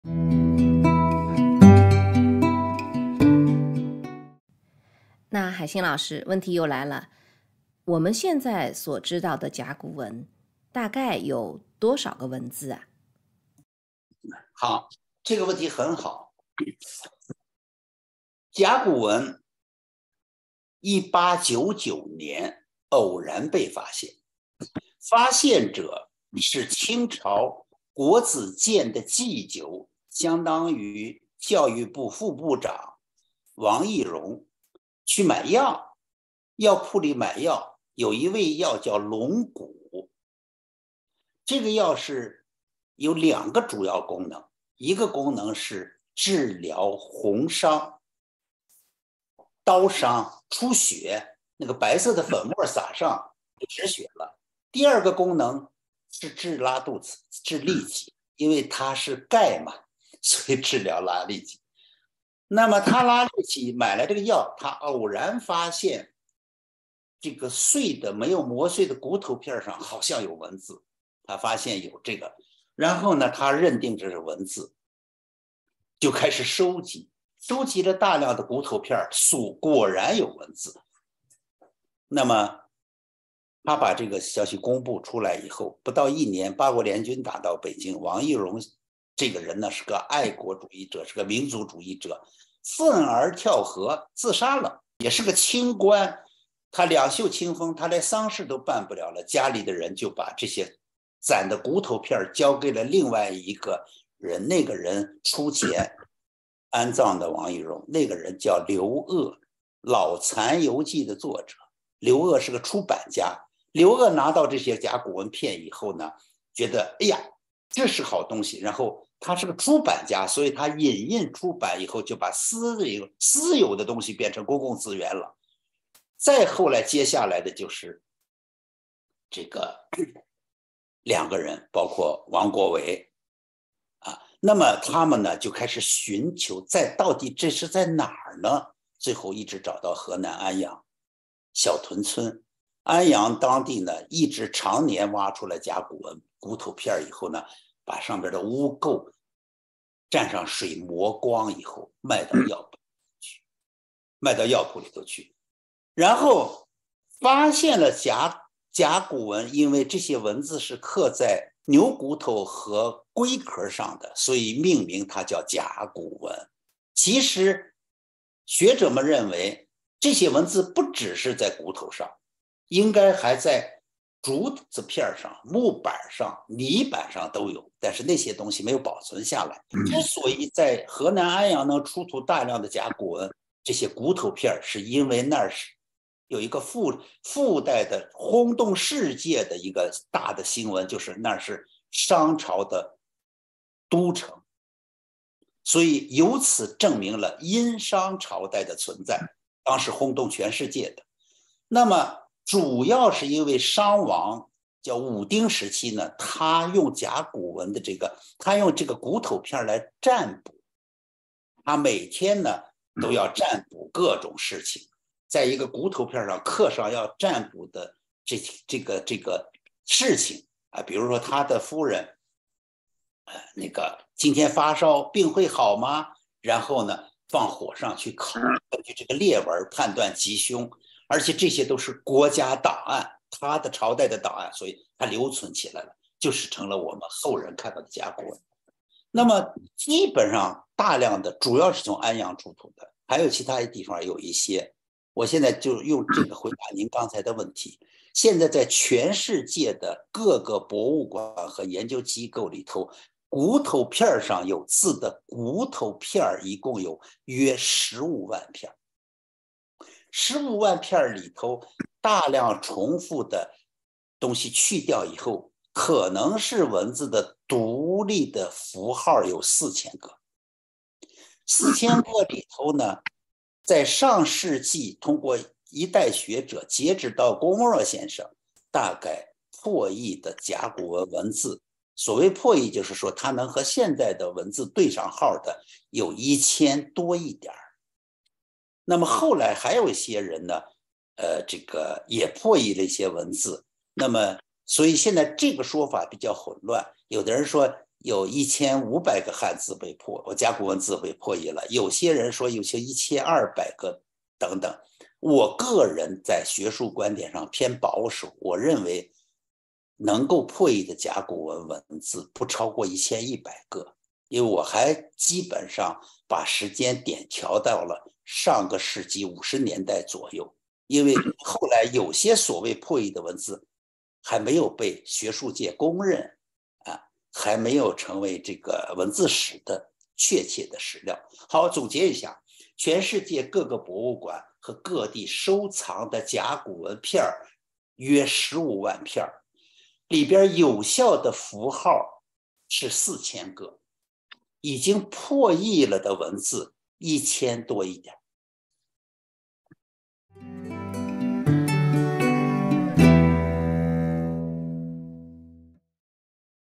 甲骨文多少文字（“书法养生养心”系列讲座之十）
2023年2月11日，著名书法艺术大师陶海心线上受访谈“书法与养生养心”。